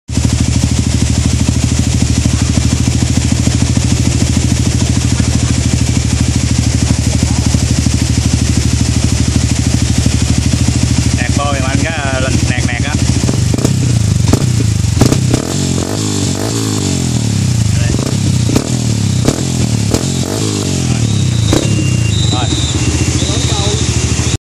Test âm thanh pô R9 trên xe Exciter 150
Mời bạn cùng thưởng thức âm thanh của pô R9 gắn trên xe Exciter 150 tại Shop Hoàng Trí. Cửa hàng chính : 158 Hàn Hải Nguyên P.8 Q.11
Chi nhánh : 586 Phạm Thế Hiển P.4 Q.8